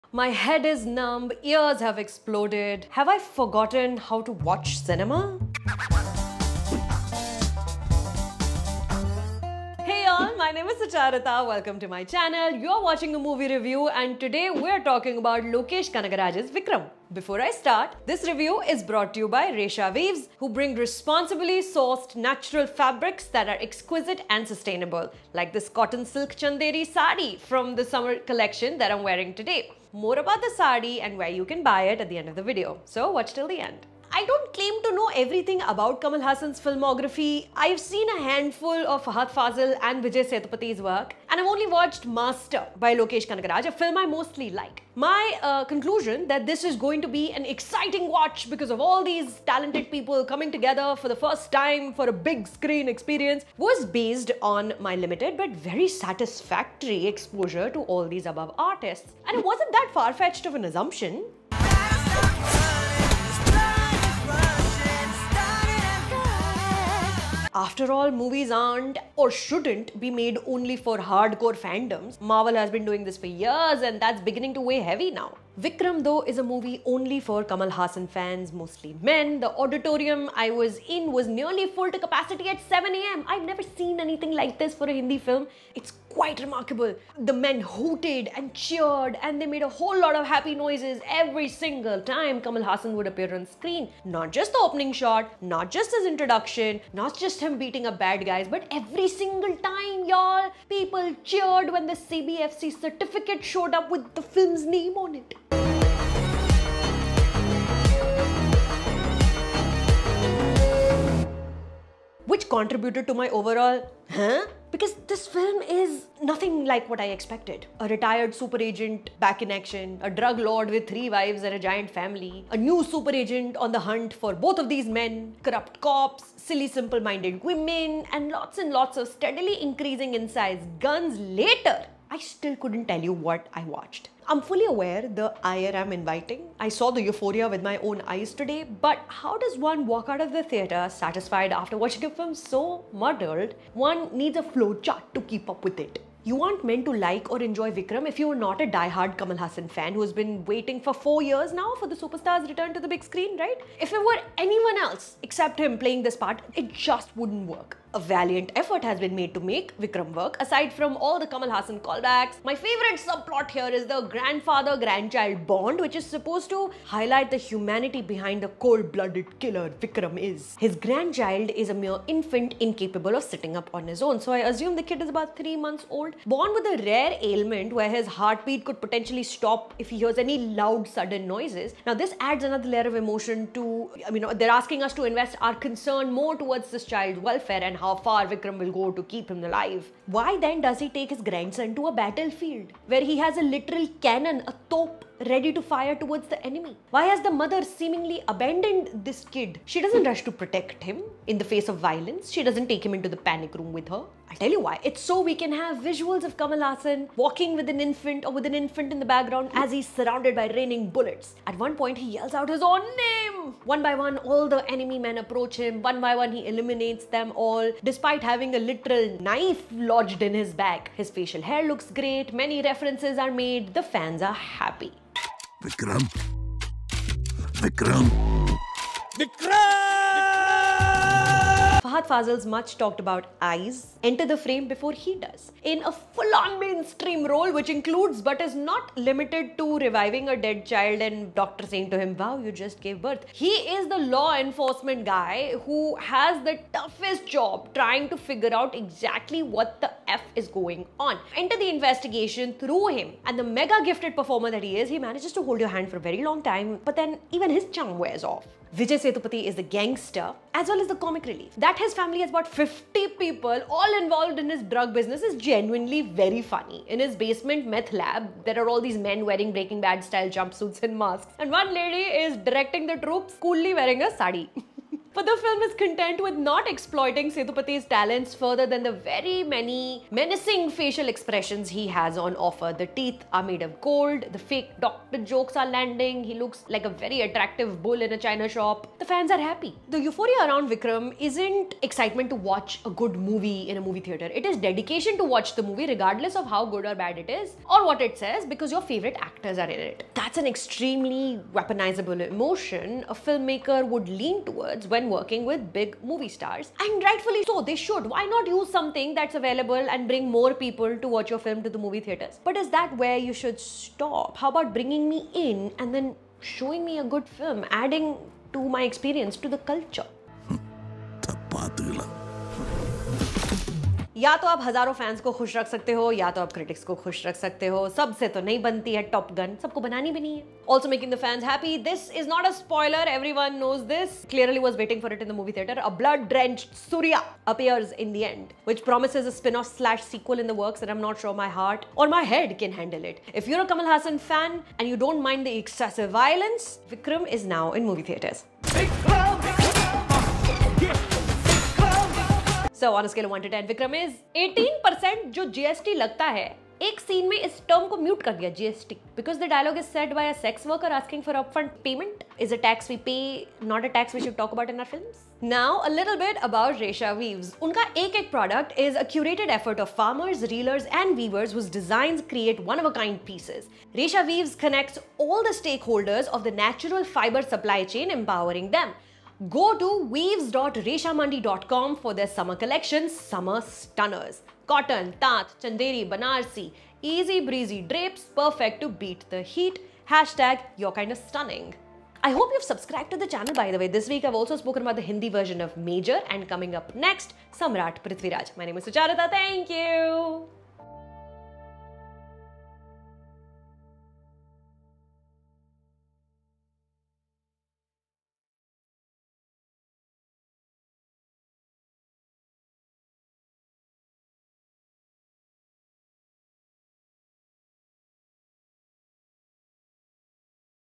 Vikram Movie Review | Sucharita Tyagi | Kamal Hassan Lokesh Kanagaraj | Tamil Movies | Hotstar
Vikram Tamil movie review by Sucharita Tyagi. Yaa toh aap hazaaron fans ko khush rakh saktey ho ya critics ko. Sabse toh nahi banti Top Gun.

The euphoria around Vikram, isn’t excitement to watch a good movie, it’s a dedication to watch the movie regardless of how good or bad it is, and what it says because your fav actor is in it. That’s an extremely weaponizable emotion a filmmaker would lean towards when working with big movie stars, and rightfully so. Why not use something that’s available, and bring more people to watch your film. But is that where you stop? How about bringing me in and then showing me a god film, adding to the culture?

Vikram is a 2022 Indian Tamil-language action thriller film written and directed by Lokesh Kanagaraj and produced by Raaj Kamal Films International. The film stars Kamal Haasan, Vijay Sethupathi and Fahadh Faasil with Kalidas Jayaram, Narain and Arjun Das in supporting roles.

Video Editor - Navin Wadhwani

Be decent people,
Sucharita